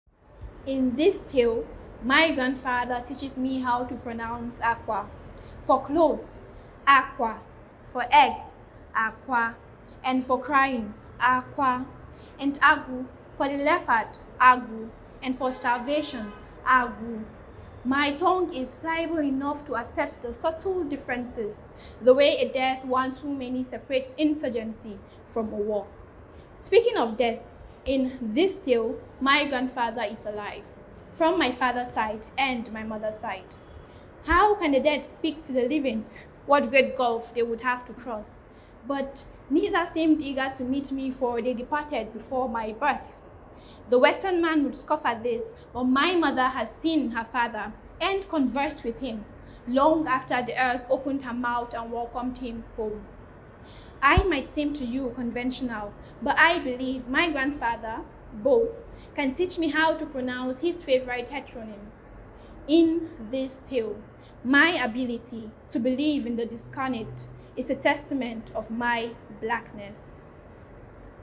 'In This Tale' by Sophia Obianamma Ofuokwu
'In This Tale' was written and performed by Sophia Obianamma Ofuokwu.

This video poem was selected from our call for poetry and spoken word. It is published on adda, our online magazine.